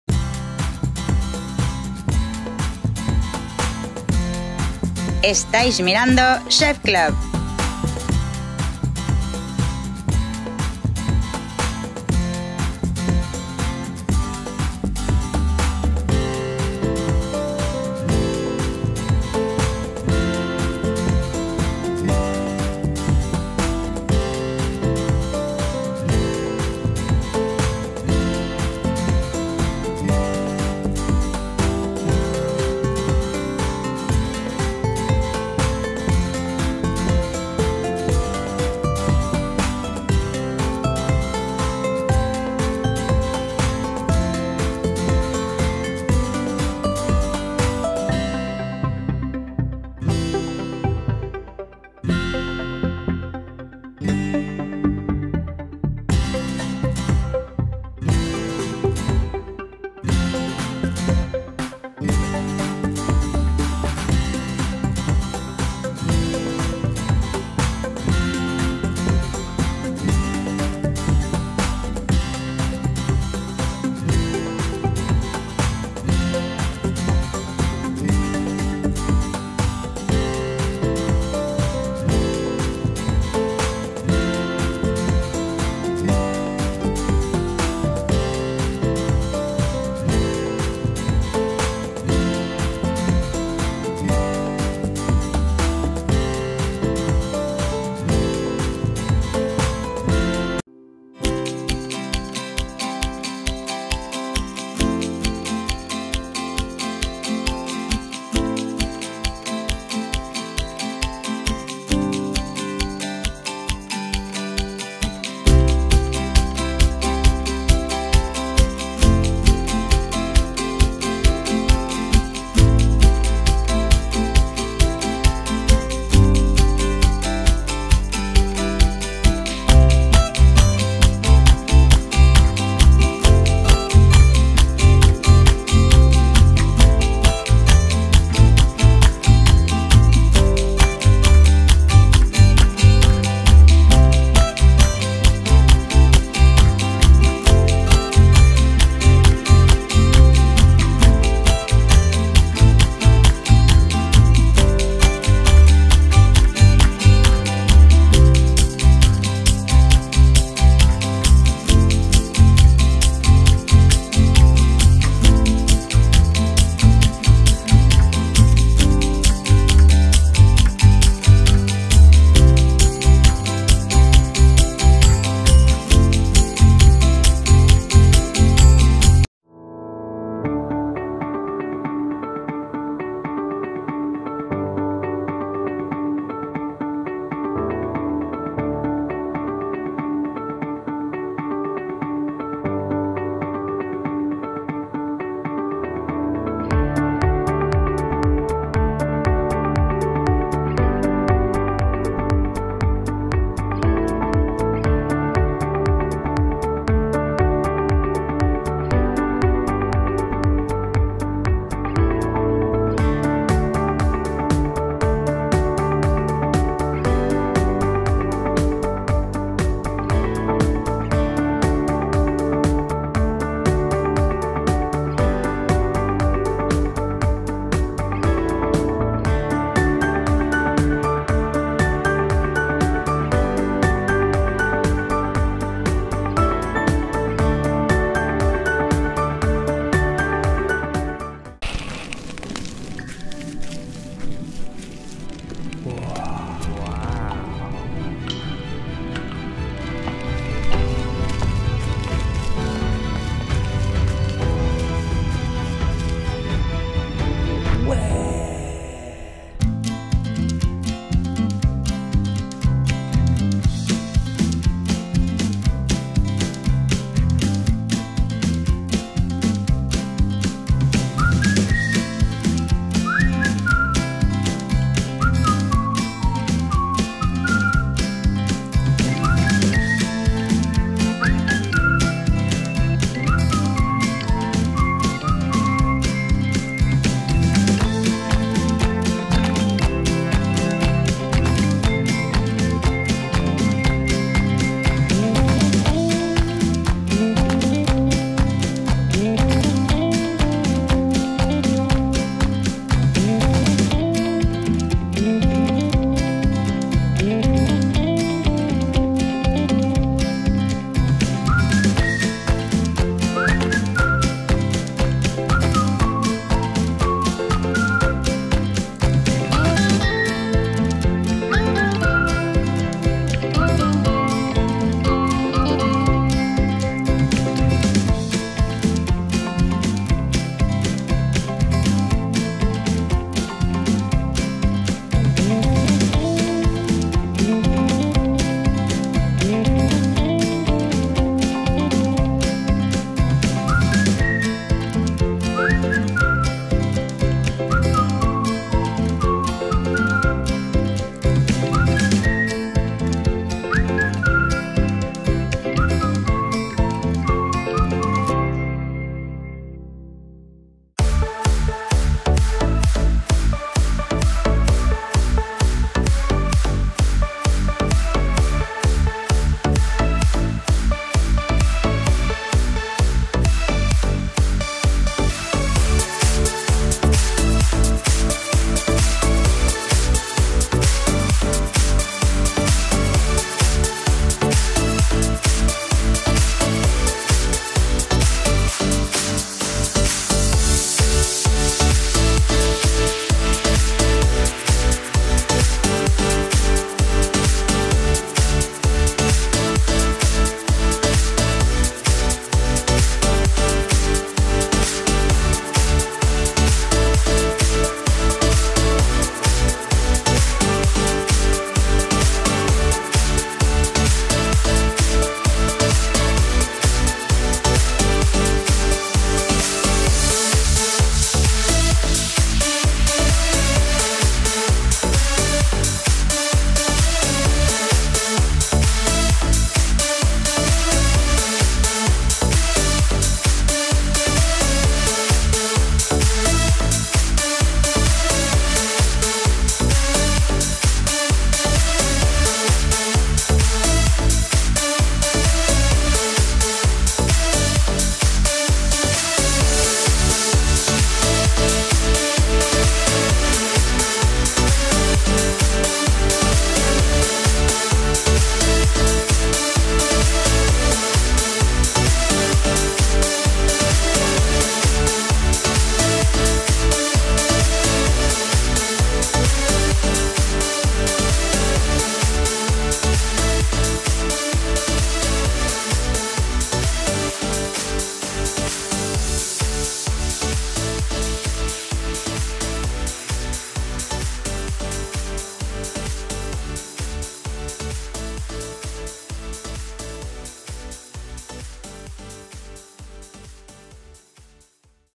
Banquete mexicano 🇲🇽 Ronda de fajitas 🌯 Tacos caseros 🌮 Torre de nachos🗼
Es la semana mexicana en Chefclub! ¡Tacos, fajitas, nachos y chiles se han unido para permitirte compartir comidas deliciosas y picantes! El mal tiempo no nos asusta, ¡estos buenos platos nos calentarán!

Pizza de jalapeño
Rollitos de pollo con guacamole
Helado de invierno
Tacos al pastor caseros
Pastel de nachos!